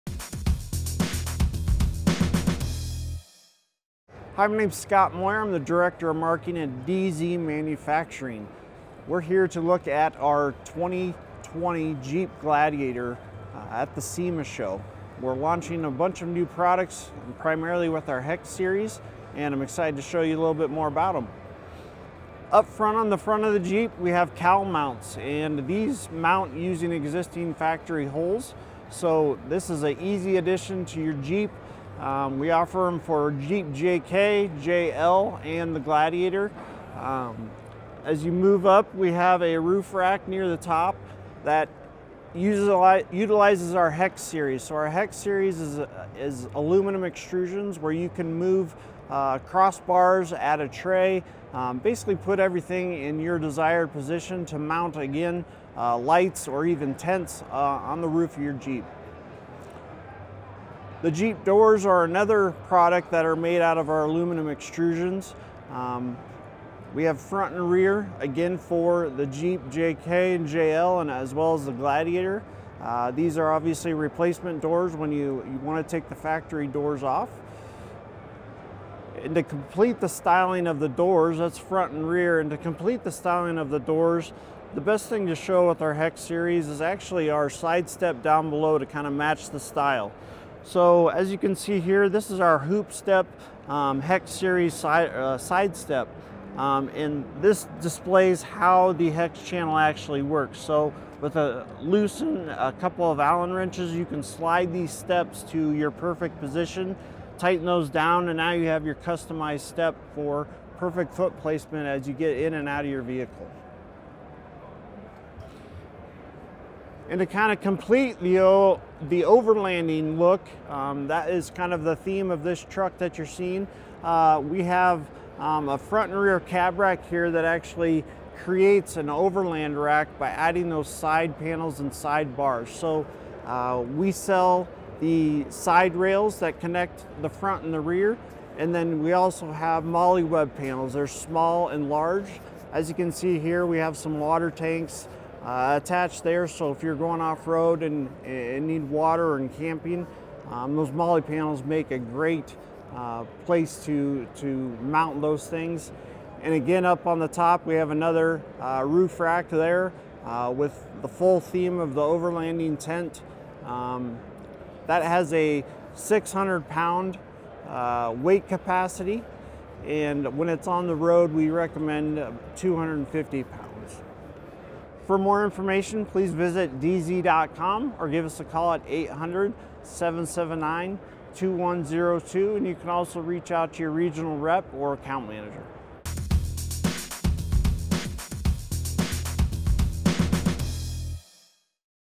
Dee Zee Mfg. at the 2019 SEMA Show
Get ready for the street or the trail with the company’s new line of Hex products for the 2020 Jeep Gladiator.